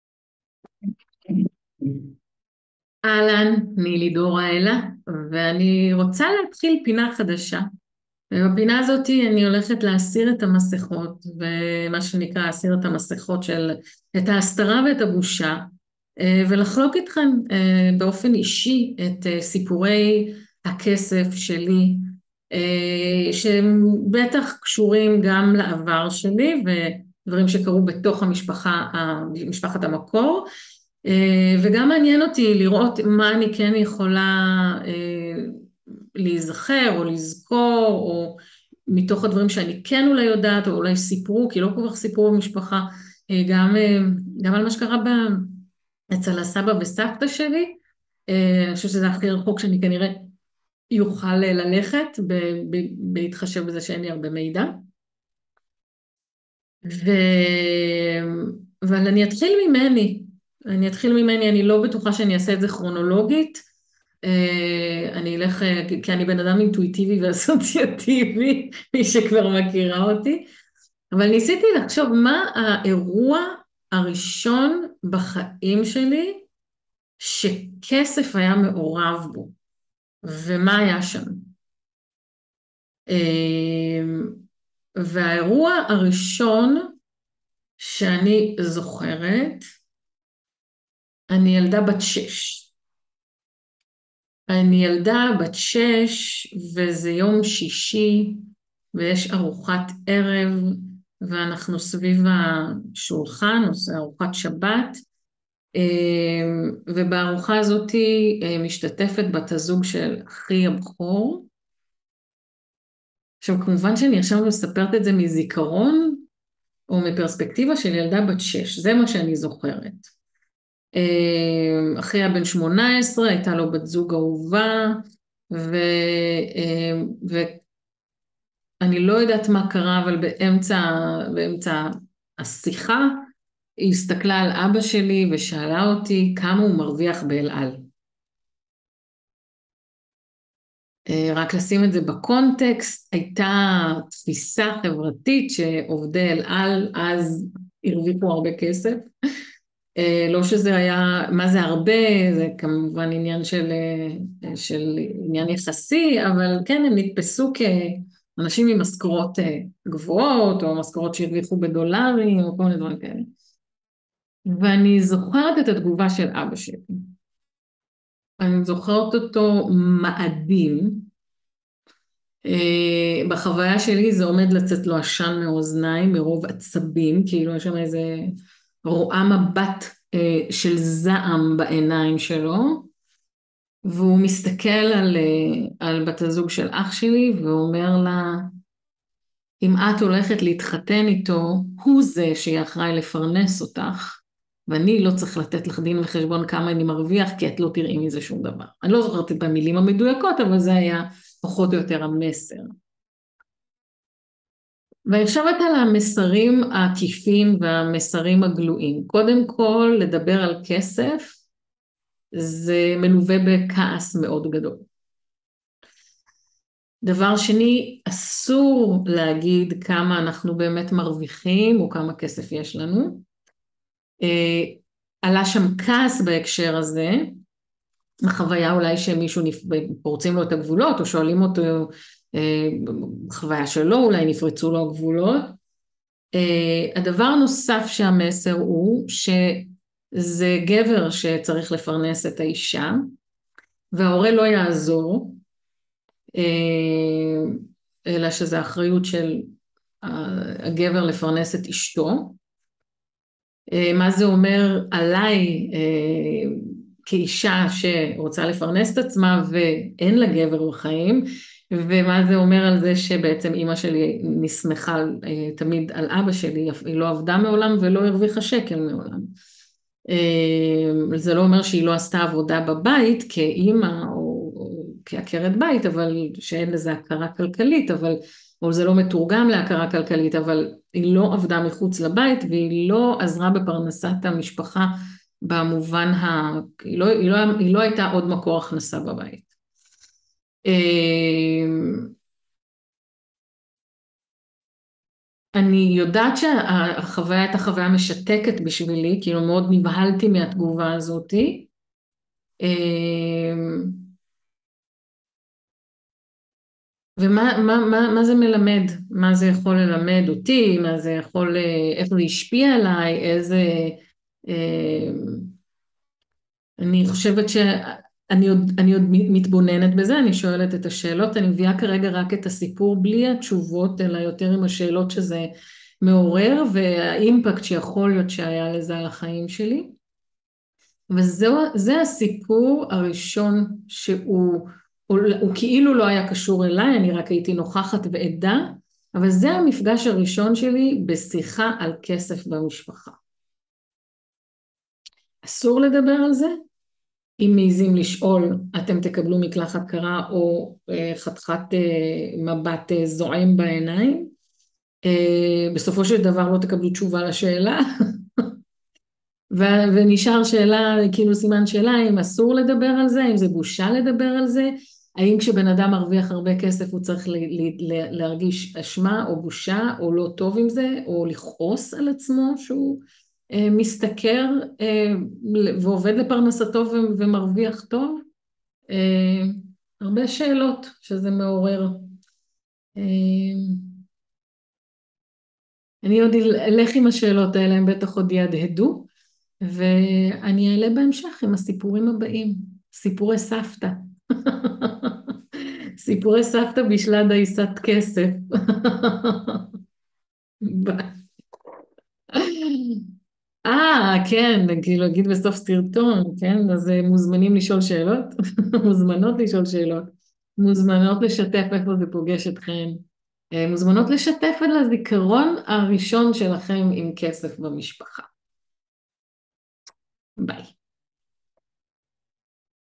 סיפורי כסף - הזיכרון הראשון שלי סביב כסף
הזיכרון הראשון שלי סביב כסף ואיזה שאלות זה מעורר אצלי.

מיידעת, מציעה ומזמינה אותך לכתוב לי
איפה זה פוגש אותך ואם בריא לך לשתף אותי
בזיכרון הראשון שלך סביב כסף.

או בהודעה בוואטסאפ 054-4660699
התייחסות אישית מובטחת.